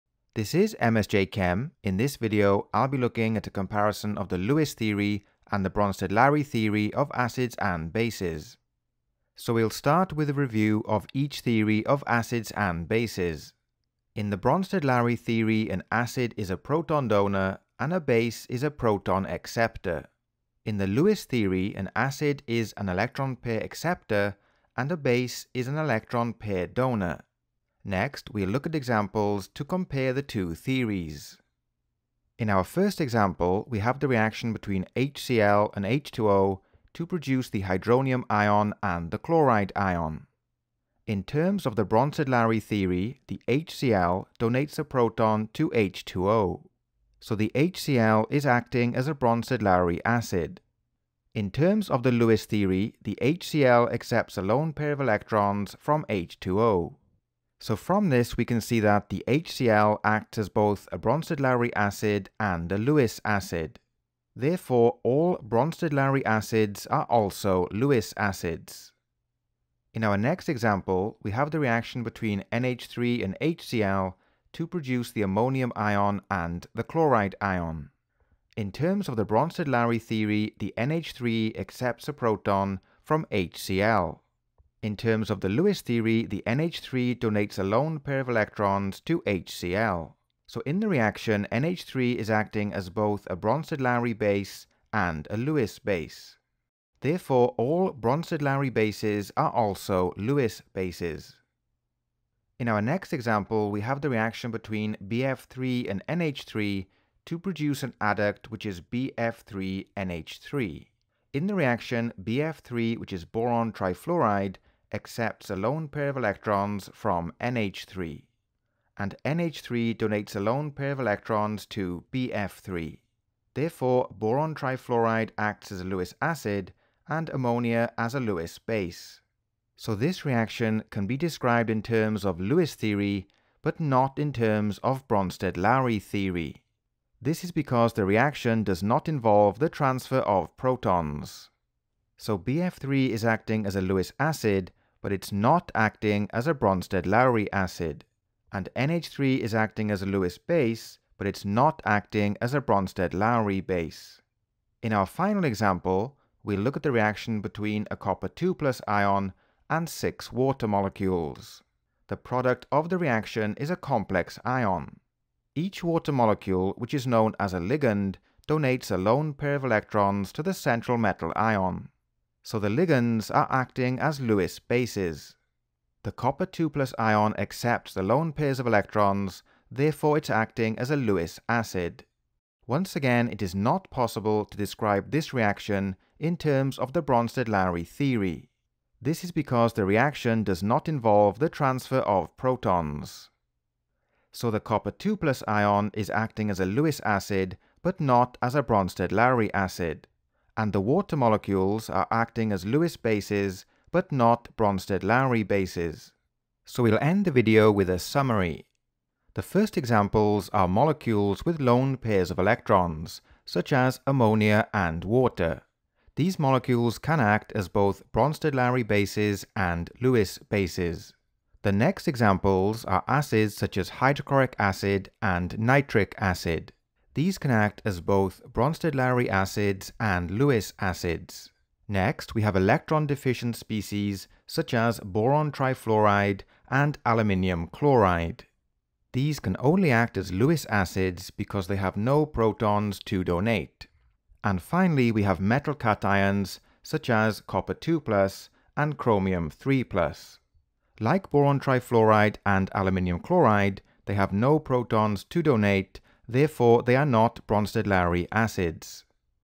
18.1 Lewis theory vs Bronsted-Lowry theory (HL)
This video is a comparison between the Lewis theory and the Bronsted-Lowry theory of acids and bases.